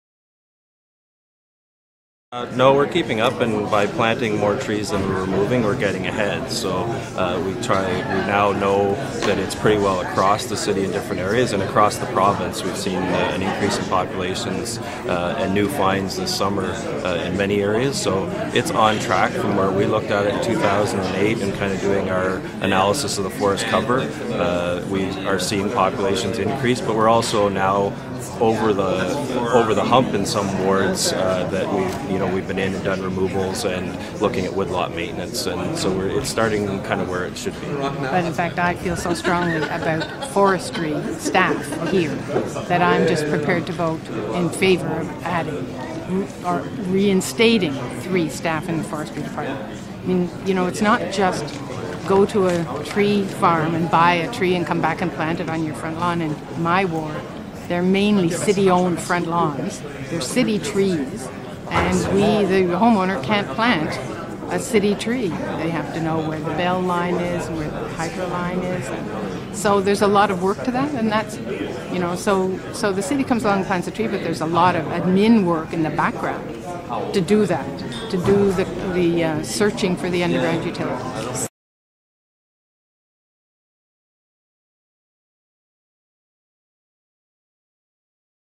Emerald Ash Borer Talk
City forestry manager David Barkley explains the fight against the emerald ash borer, while Somerset Coun. Diane Holmes explains why she wants more funding for forestry services.